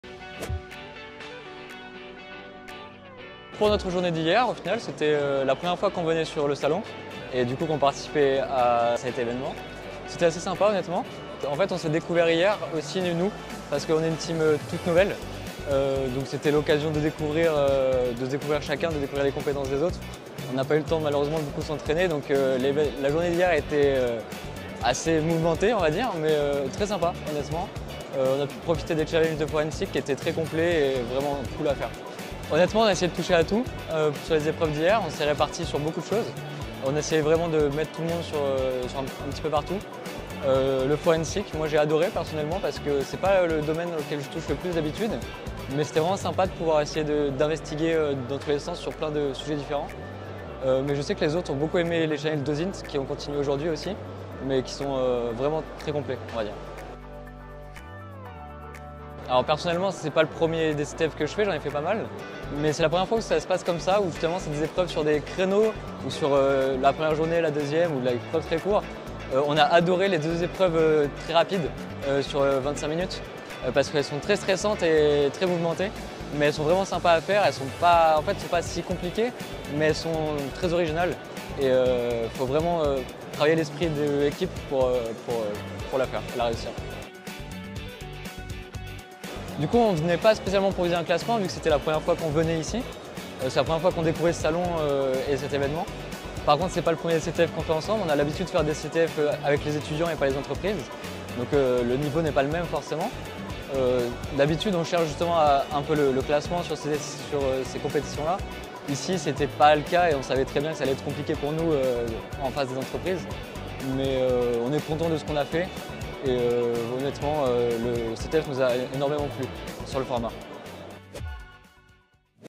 Nos étudiants "AtlantHack" à l'European Cyber Cup
👏Une première participation de nos étudiants à l'EuCyberCup !